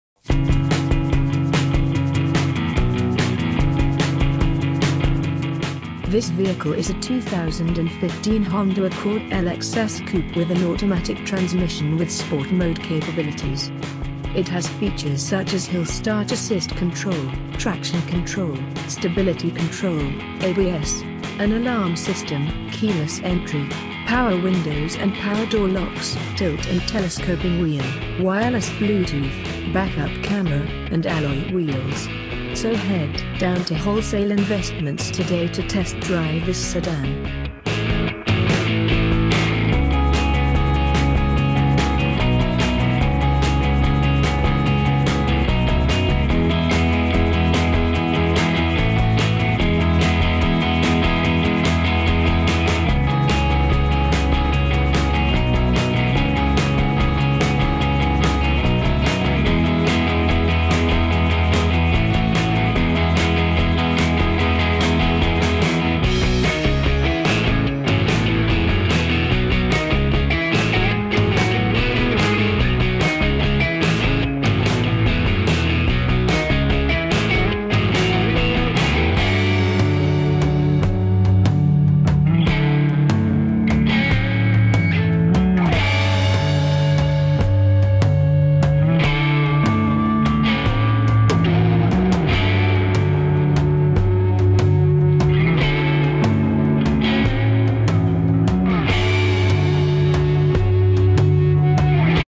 2015 Honda Accord LX-S - Our Perspective
This vehicle is a 2015 Honda Accord LX-S Coupe with an automatic transmission with sport mode capabilities. It has features such as Hill Start Assist Control, traction control, stability control, ABS, an alarm system, keyless entry, power windows and power door locks, tilt and telescoping wheel, wireless Bluetooth, backup camera, and alloy wheels. So head down to Wholesale Investments today to test drive this sedan.